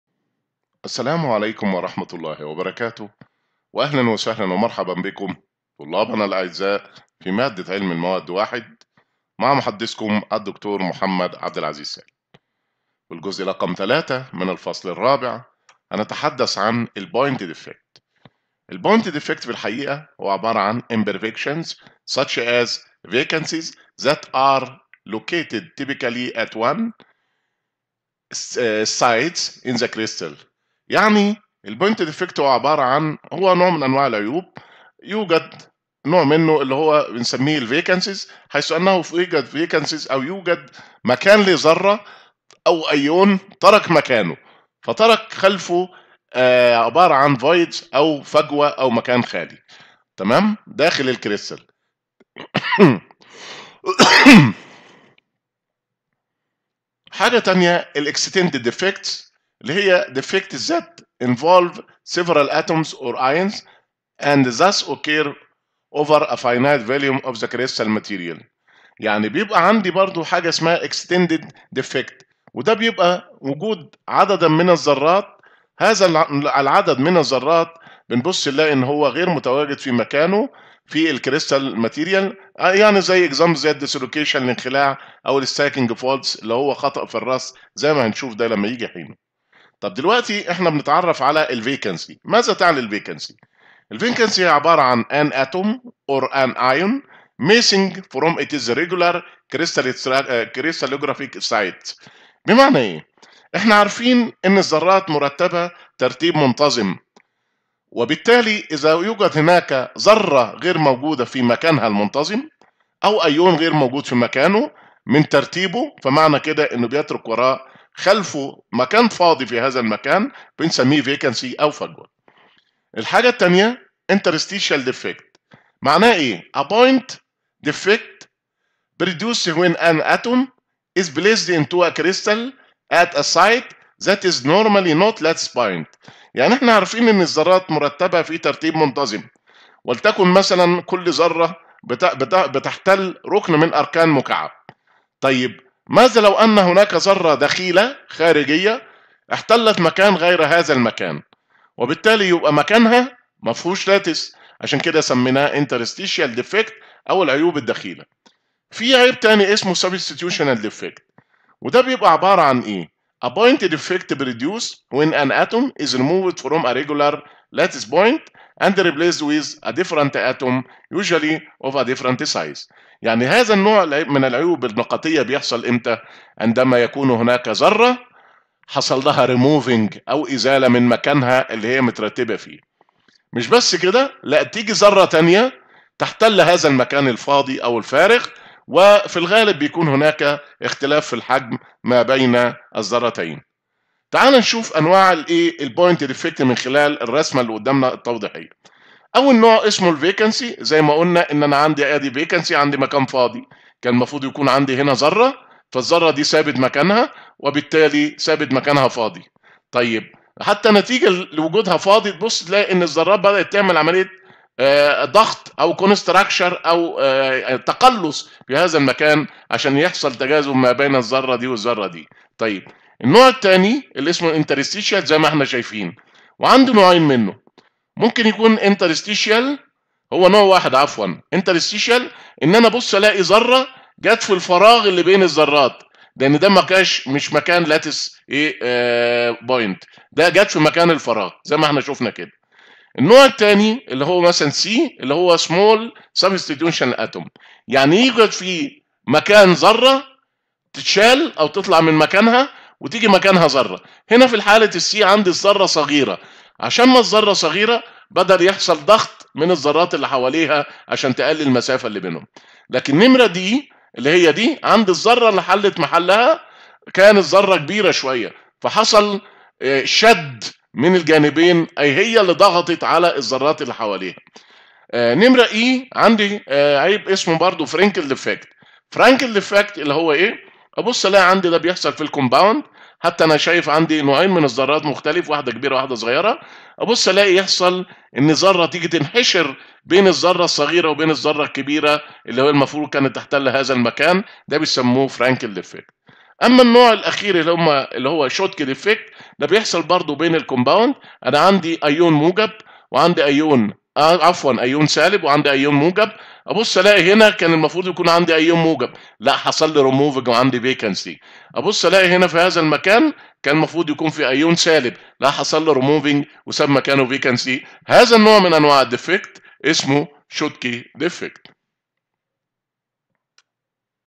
Material Science - 1 | C4-L3 | Point defect
Chapter 4 : Crystal imperfection and atom movement
Lecture 3 : Point defect
Explain the types of point defect